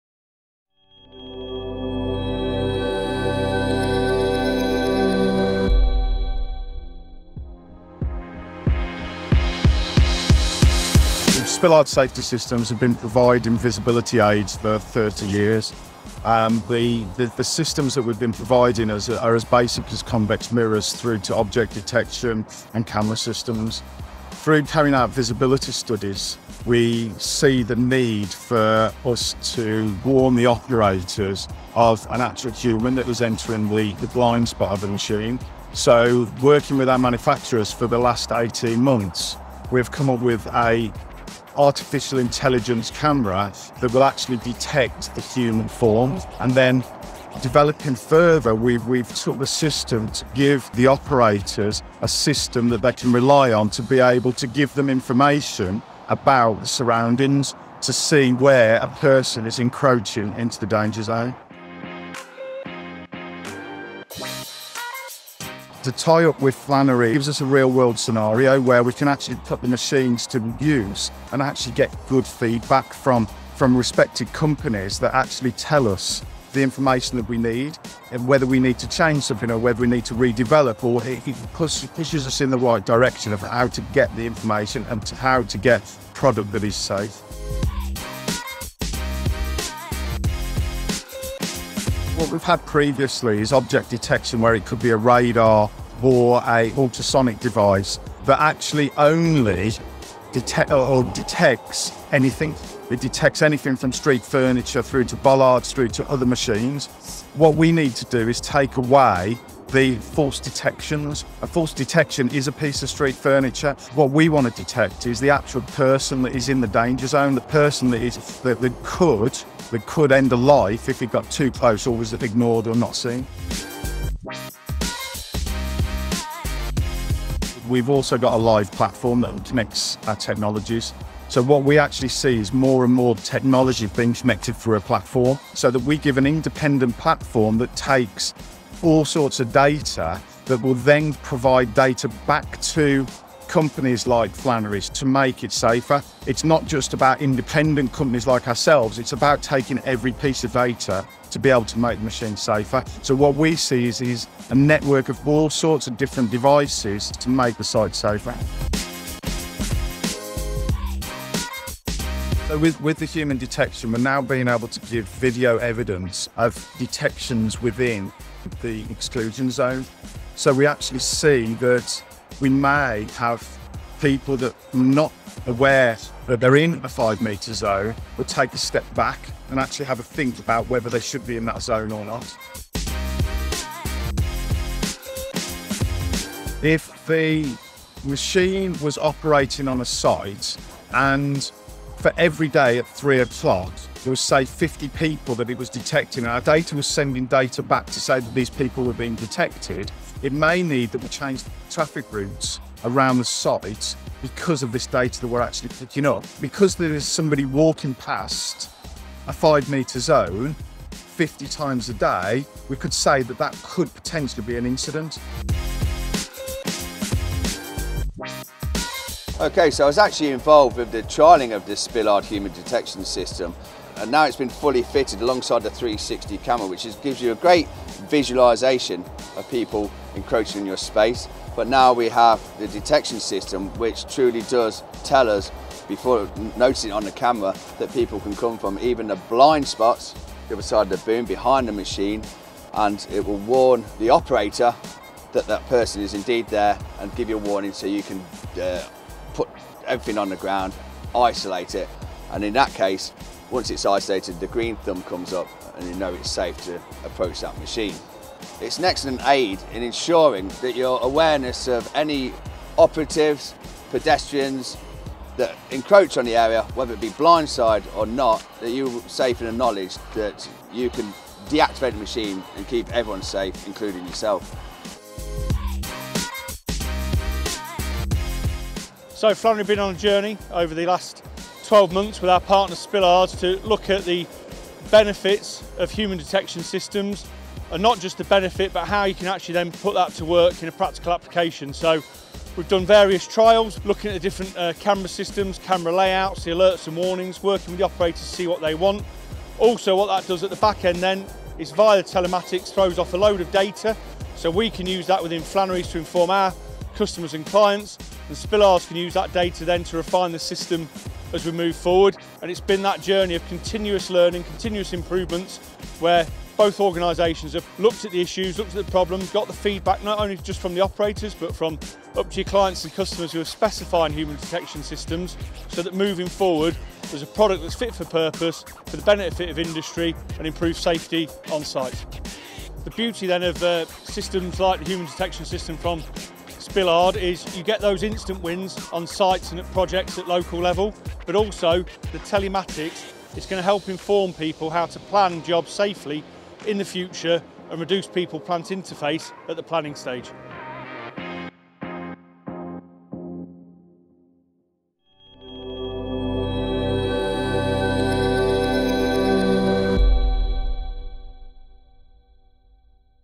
The Spillard Human Detection System
Flannery have been working closely with Spillard Safety Systems to develop and launch the latest Human Detection System on our fleet.

Find out more about this safety innovation and how it can help with the risks associated with people plant interface as we catch up with Pete Spillard to showcase the new technology.